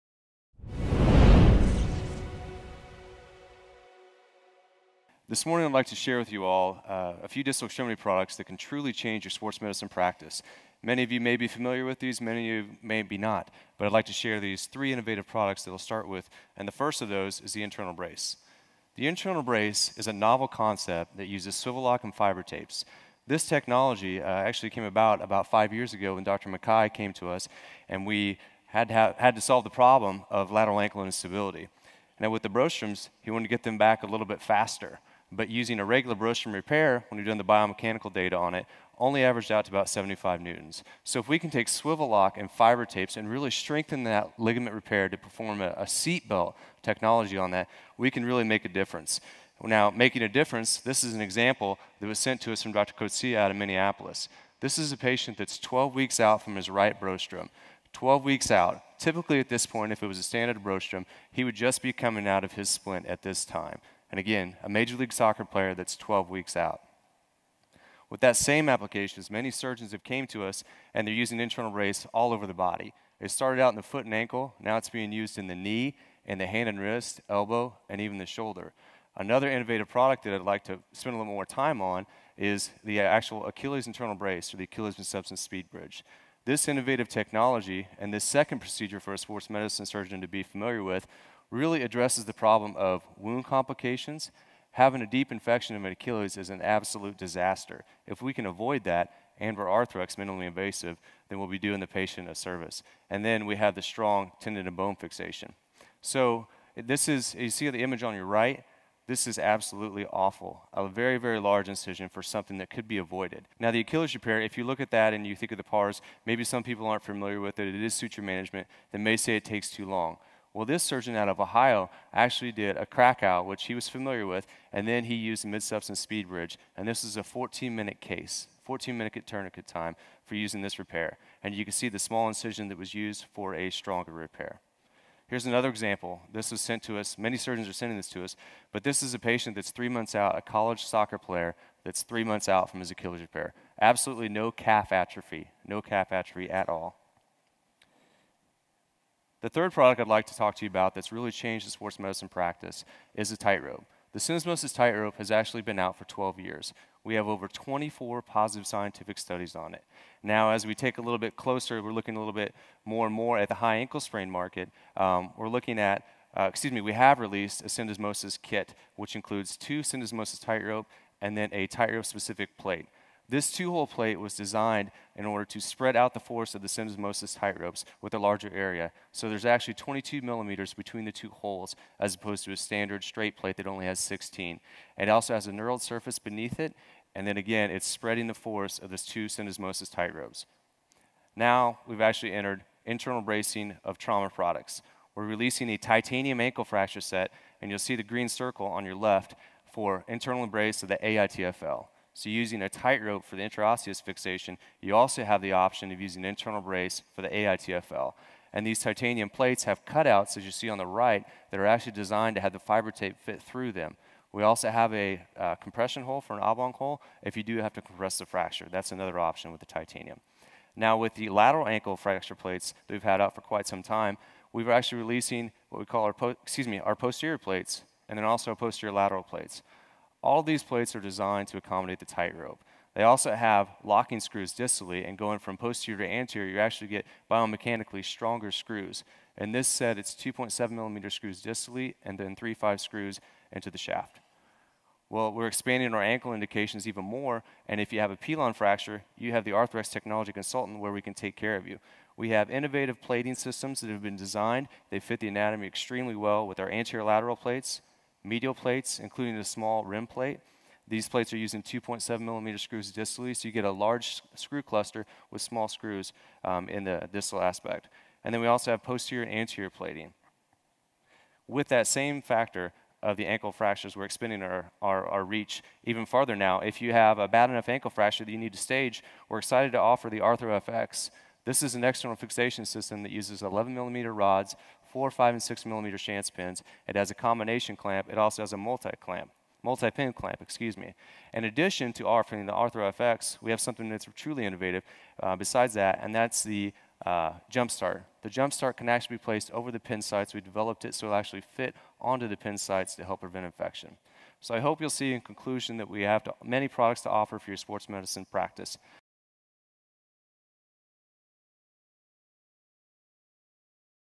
Innovations in Foot and Ankle Trauma for the Sports Surgeon
Todd Earhart (Arthrex® Product Manager, Distal Extremities) presents innovations in foot and ankle trauma. He discusses surgical treatment solutions for lateral ankle, Achilles tendon, syndesmosis and lower extremity fractures.